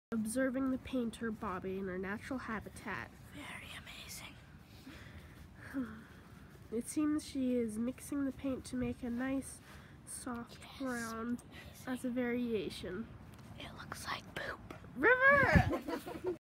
Painting mural at Corning Library
In spring 2018, myself and several students from the art department at California State University Chico embarked on a journey to bring several murals into our local community.